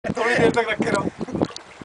Vikki in Qatar
Vivek enjoys climbing on anything he can, after landing here in Qatar there has been a drastic change in his taste  ;D